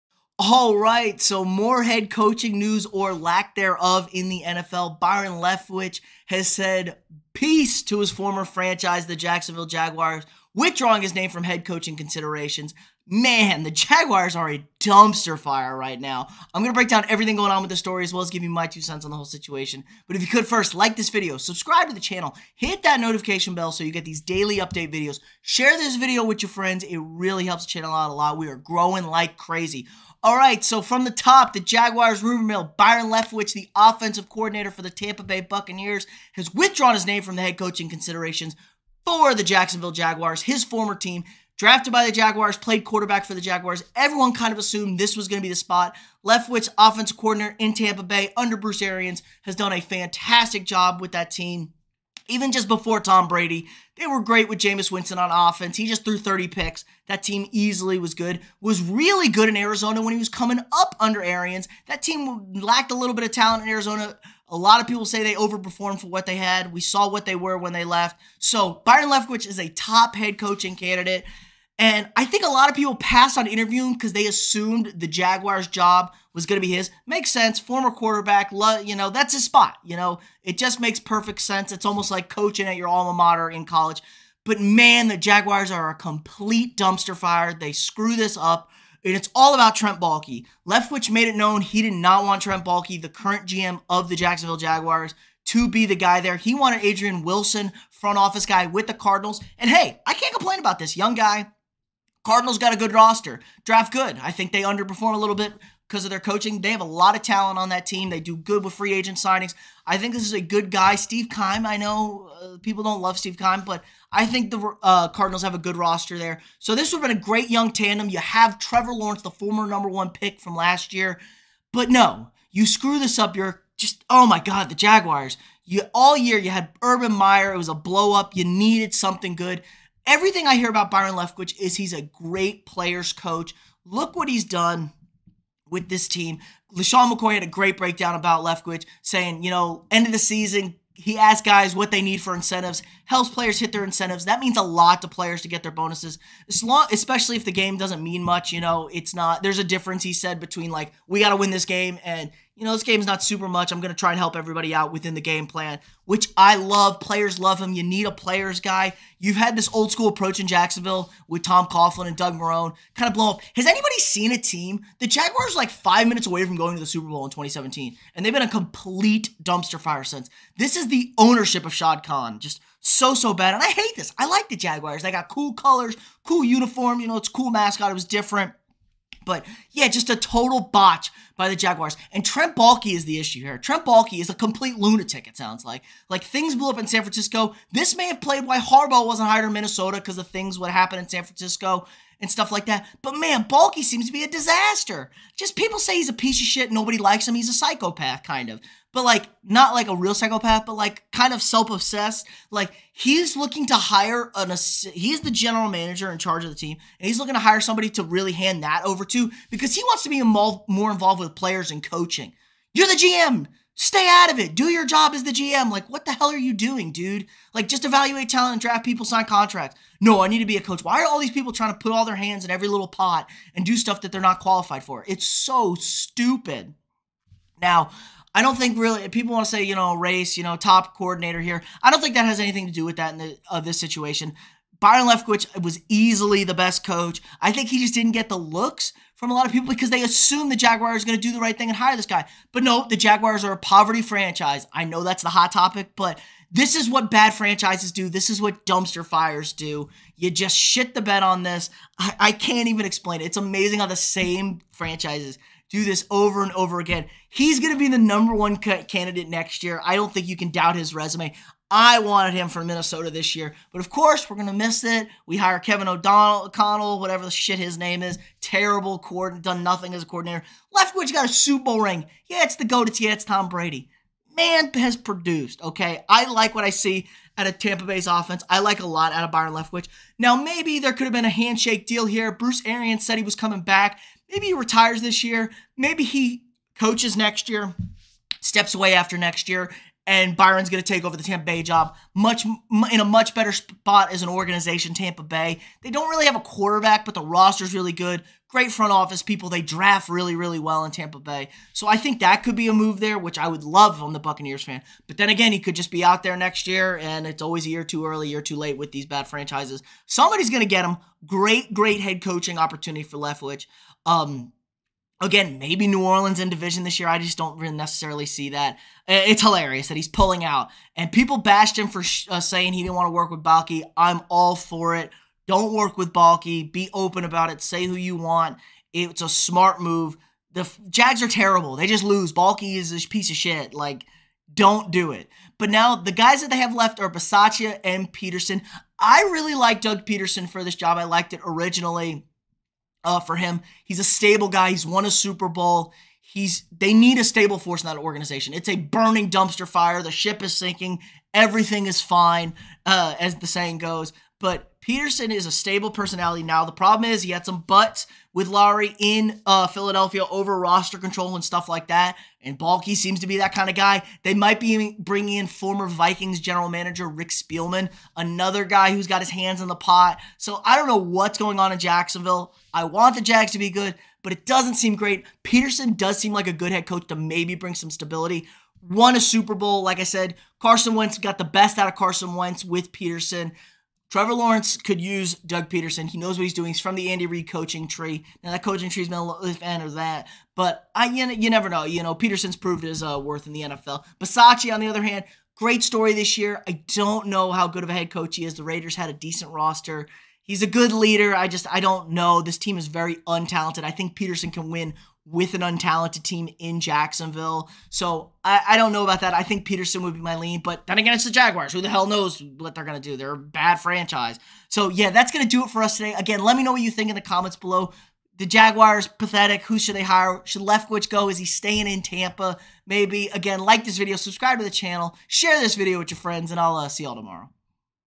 Byron Leftwich Withdraws from Jaguars HC Job. Jacksonville is a Dumpster Fire!
Check Us Out at:

@AfterReviewPT- Twitter
Apple Podcasts: After the Review